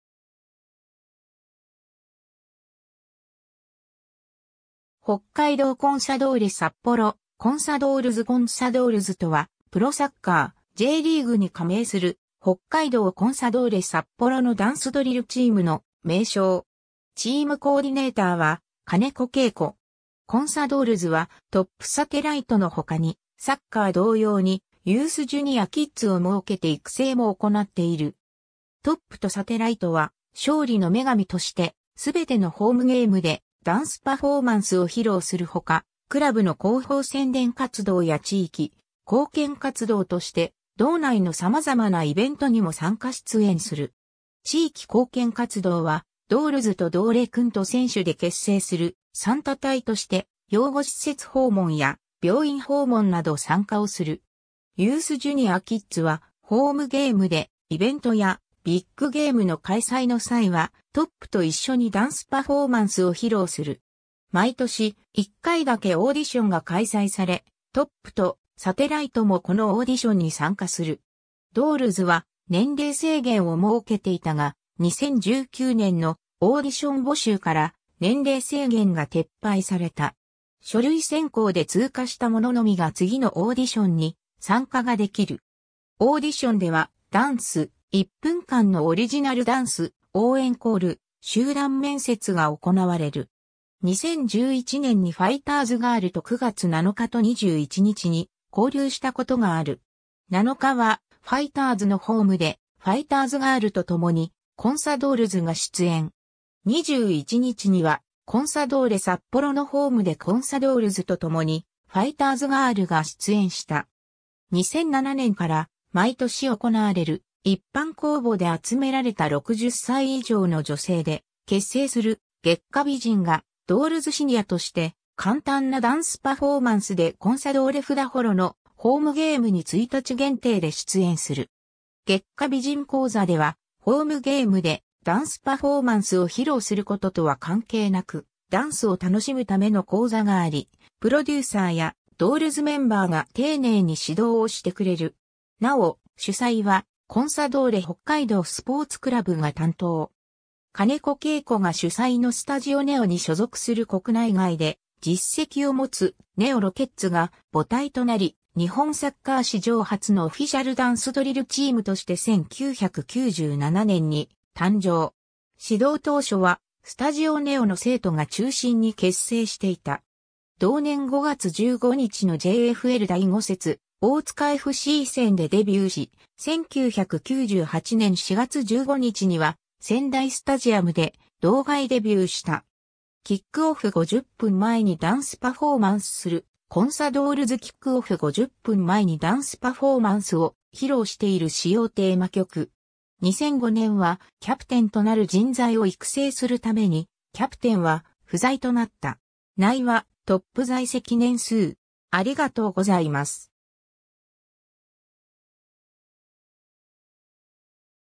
コンサドールズ
北海道コンサドーレ札幌 」 コンサドールズ コンサドールズ（Consadolls）とは、プロサッカー・Jリーグに加盟する北海道コンサドーレ札幌のダンスドリルチームの名称。
チームコーディネーターは金子桂子。
 コンサドールズはトップ・サテライトの他に、サッカー同様にユース(中高生)・ジュニア(小中学生)・キッズ(園児)を設けて育成も行っている。
トップとサテライトは『勝利の女神』として全てのホームゲームでダンスパフォーマンスを披露する他、クラブの広報宣伝活動や地域貢献活動として、道内の様々なイベントにも参加出演する。
地域貢献活動はドールズとドーレくんと選手で結成する『サンタ隊』として、養護施設訪問や病院訪問(小児科)など参加をする。
ユース・ジュニア・キッズはホームゲームでイベントやビッグゲームの開催の際はトップと一緒にダンスパフォーマンスを披露する。
 毎年、1回だけオーディションが開催され、トップとサテライトもこのオーディションに参加する。
ドールズは年齢制限(25歳まで)を設けていたが、2019年のオーディション募集から年齢制限が撤廃された。
書類選考で通過した者のみが次のオーディションに参加ができる。
オーディションではダンス(基礎、基本動作)、1分間のオリジナルダンス(選曲自由)、応援コール、集団面接が行われる。
 2011年にファイターズガール(北海道日本ハムファイターズのチアリーディング)と9月7日と21日に交流したことがある。
7日はファイターズのホームでファイターズガールと共にコンサドールズが出演。
21日にはコンサドーレ札幌のホームでコンサドールズと共にファイターズガールが出演した。
 2007年から毎年行われる一般公募で集められた60歳以上の女性で結成する『月下美人』がドールズシニアとして簡単なダンスパフォーマンスでコンサドーレ札幌のホームゲームに一日限定で出演する。
月下美人講座では、ホームゲームでダンスパフォーマンスを披露することとは関係なく、ダンスを楽しむための講座があり、プロデューサーやドールズメンバーが丁寧に指導をしてくれる。
なお、主催はコンサドーレ北海道スポーツクラブが担当。
 金子桂子が主宰のスタジオNEOに所属する国内外で実績を持つ『NEOロケッツ』が母体となり、日本サッカー史上初のオフィシャルダンスドリルチームとして1997年に誕生。
始動当初はスタジオNEOの生徒が中心に結成していた。
同年5月15日のJFL第5節大塚FC戦(札幌厚別)でデビューし、1998年4月15日には仙台スタジアム（浦和戦）で道外デビューした。
 キックオフ50分前にダンスパフォーマンスするコンサドールズ（2014年4月29日） キックオフ50分前にダンスパフォーマンスを披露している使用テーマ曲。
 2005年はキャプテンとなる人材を育成するためにキャプテンは不在となった。
 （）内はトップ在籍年数